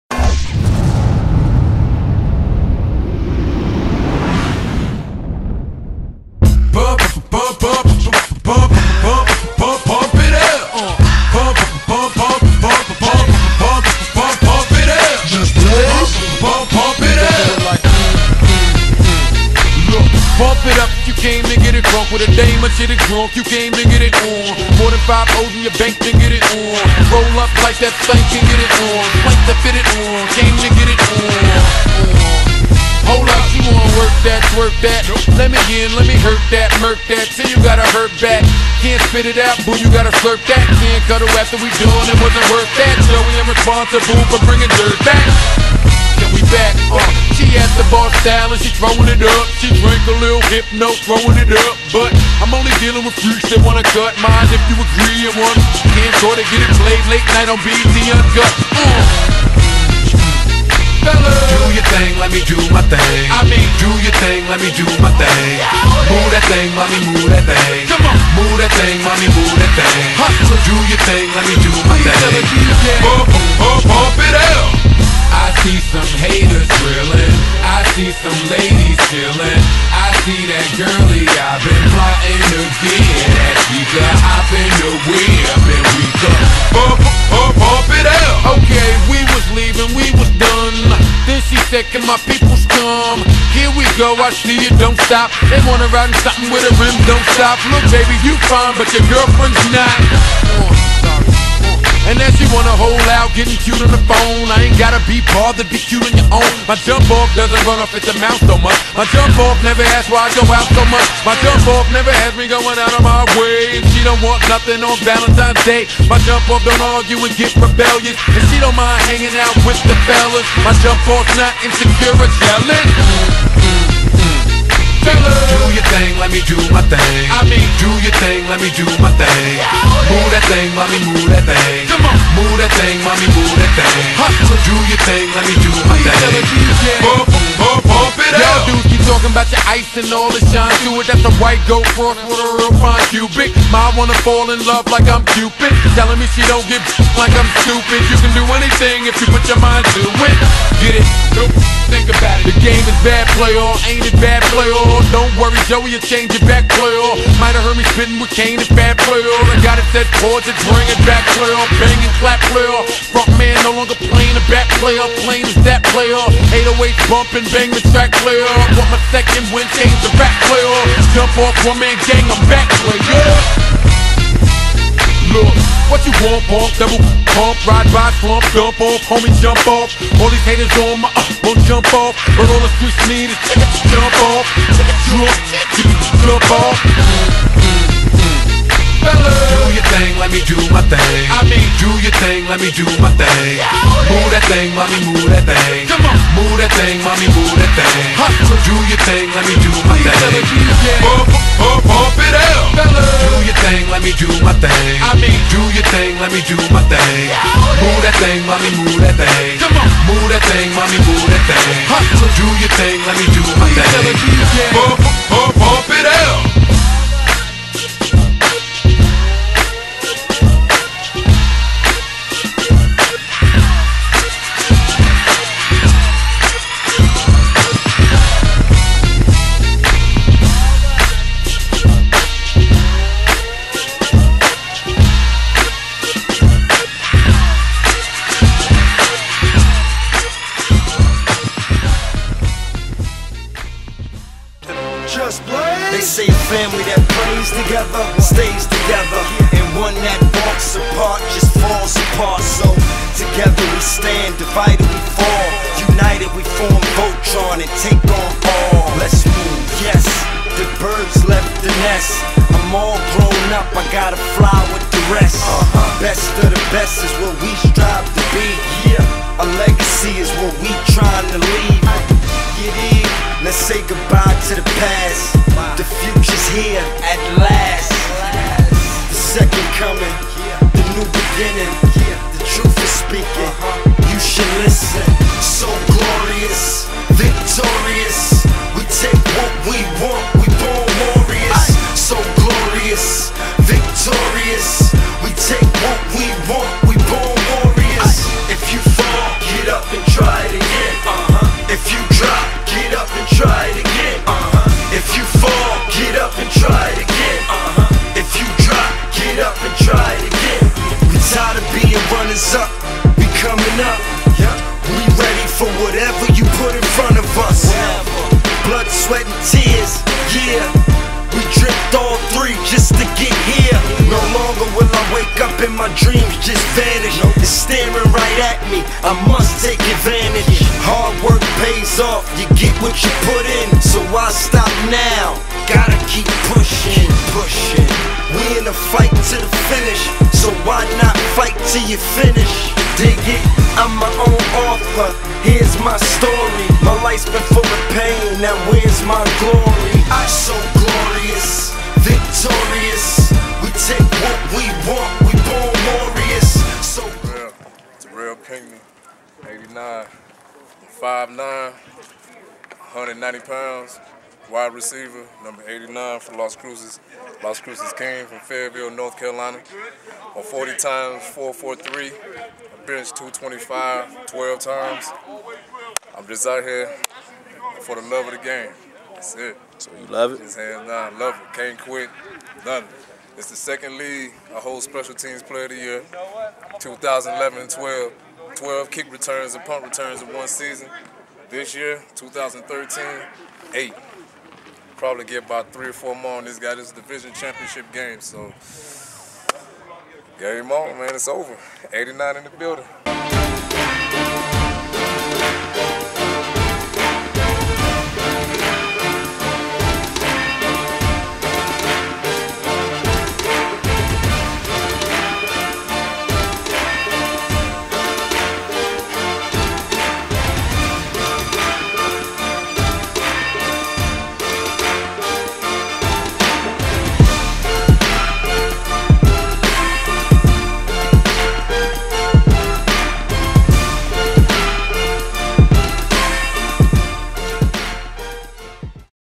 #89 Terrell Pinckney - Special Teams Player of the Year 2013 KR/PR/WR
What's going on everyone. I'm Terrell Pinckney 5'9-190lbs WR/KR/PR on the western side of TX and NM. Born and raised in NC. I been ready for the next level and always will be. It doesn't matter what team in what league, Ill play for anyone for the love of the game. I love the game so much, who ever interested in me, Ill pay for everything myself for whatever. NFL, CFL, or AFL ill play for free. Just to show another piece of my talent I went to another team in another league and gotten the same results. Within 2 years in 2 different football leagues in 2 different states, Texas and New Mexico, I've earned Special Teams Player of the Year. Total of 20 KR/PR for touchdowns. So, what's next peoples of Youtube. I've played against numerous of players from all over the country and ends with the same results. People, coaches, and players always ask how do I return. Honestly I have a unknown secret to getting great kick/punt returns. trust me, you'll never know.